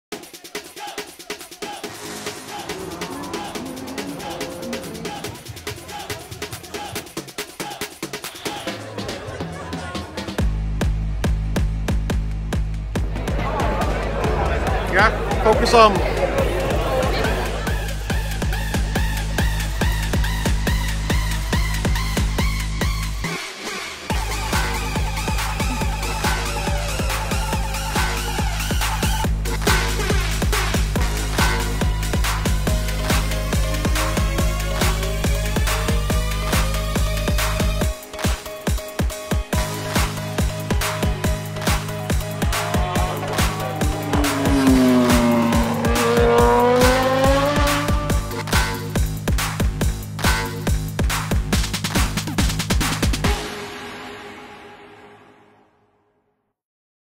Isle of Man 2018 Memories
Spur Of The Moment Thing But Friday 8th June 2018 Was An Amazing Experience.. It Was Sunny ☀️ Tiring (someone nodded of standing up) Atmosphere Fantastic .Thankyou 😜👍😜